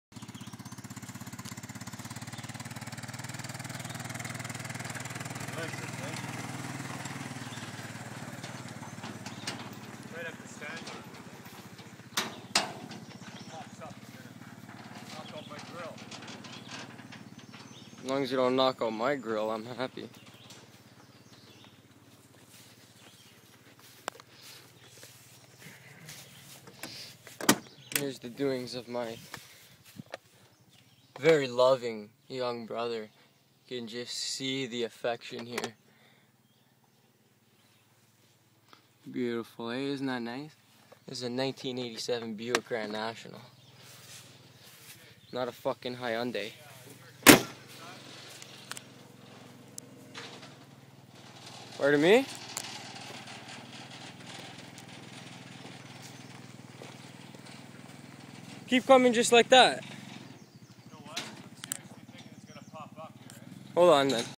Its a Buick not a Hyundai!
this is what happens when someone doesn't know how to take out their anger. he's lucky to be alive